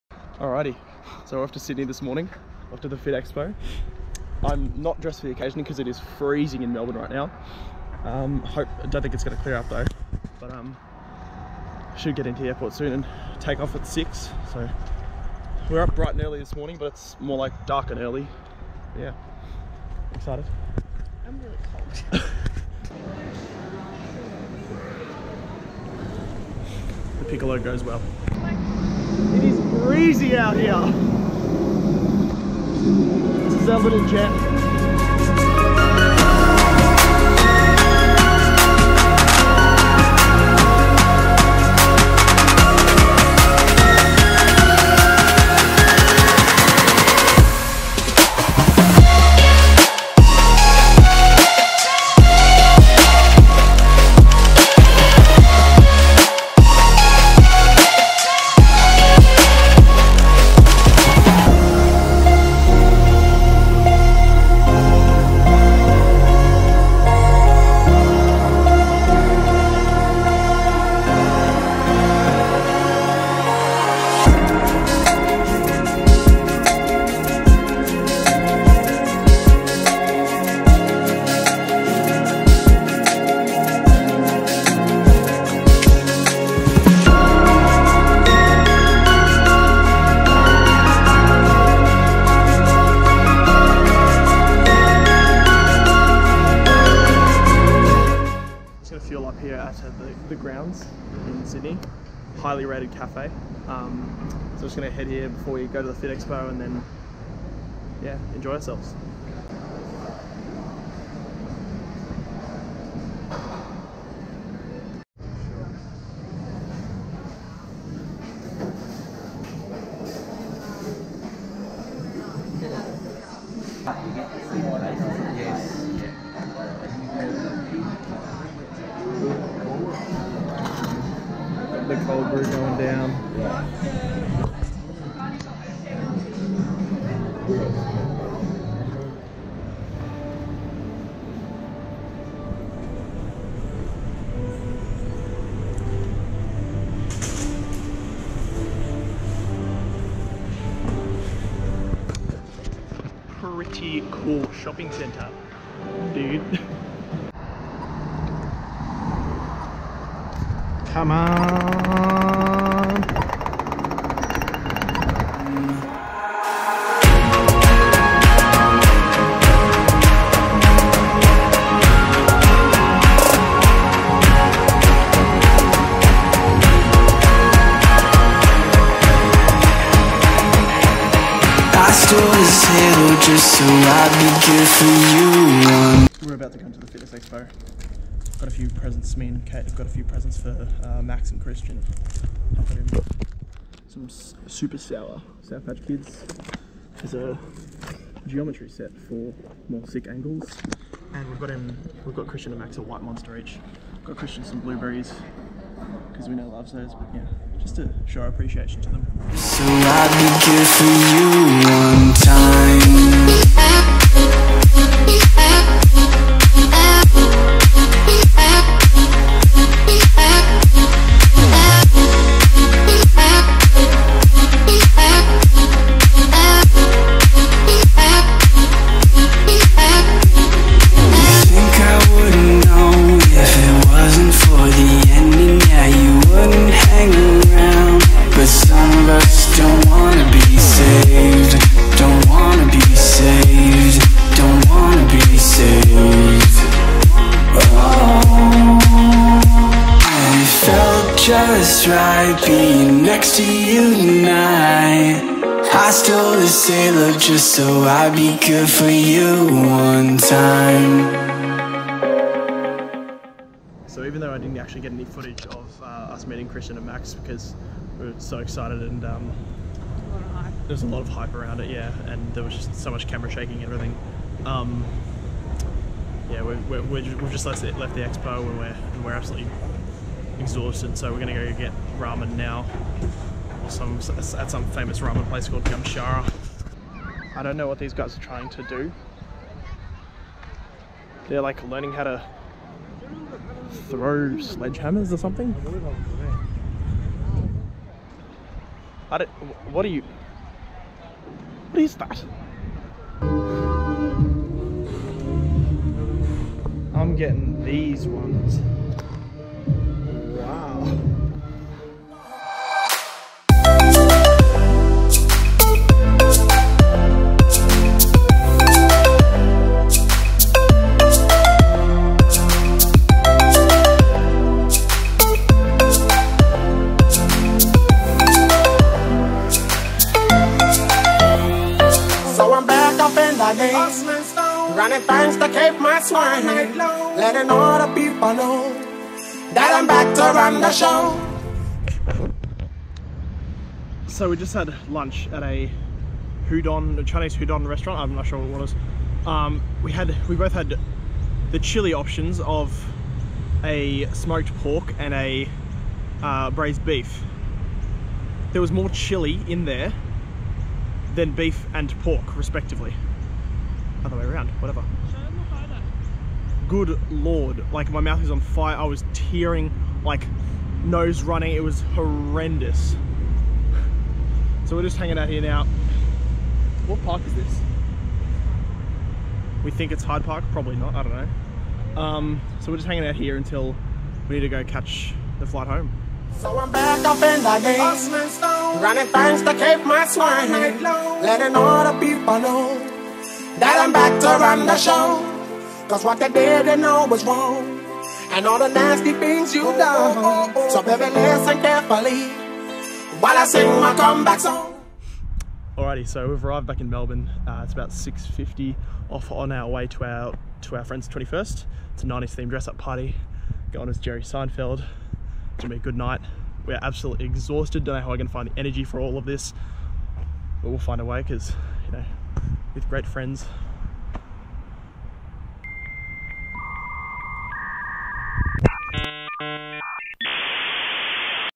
Sydney Fitness Expo - MY FIRST VIDEO
My first YouTube video :D
Really enjoyed learning how to make this.
Sorry for some of the jittery footage, will work on this in the future.